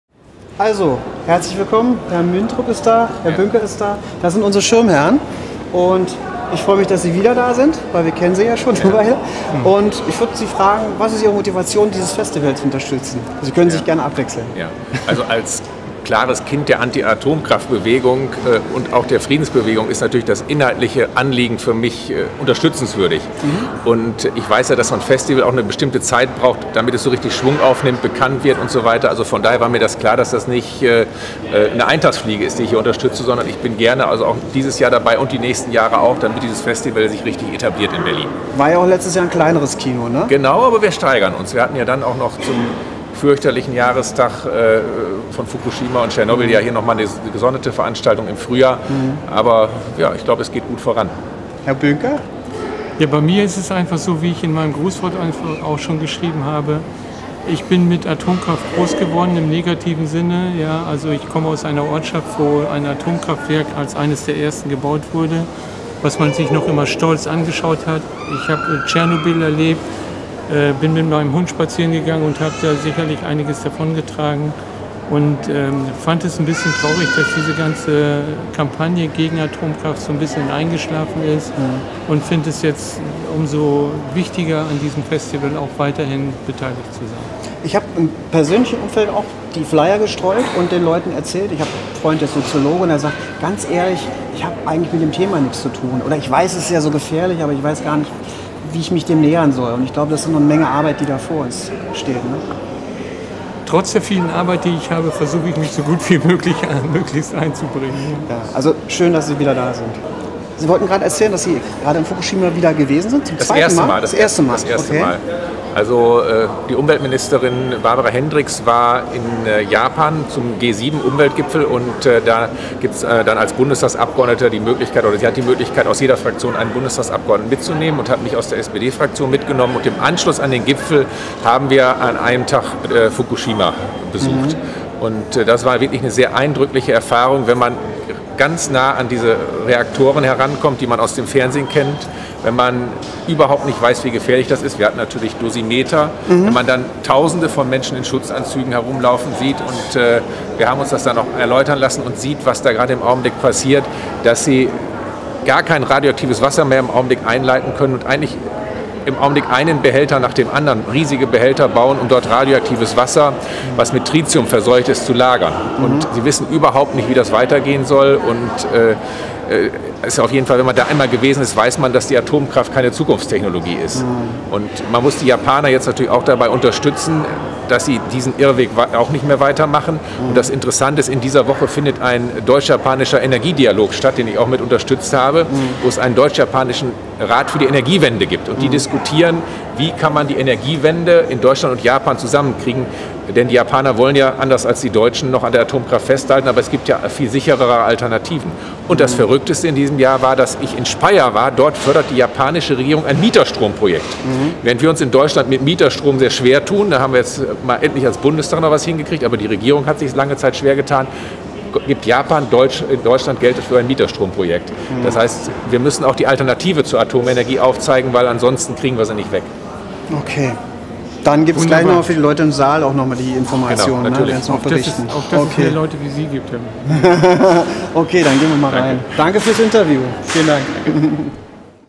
MdB Kaus Mindrup ... zur Eroeffnung des Uranium Film Festivals in Berlin 2016
Interview mit Klaus Mindrup MdB und Uwe Bünker ueber Atomkraft und das Internationale Uranium Film Festival.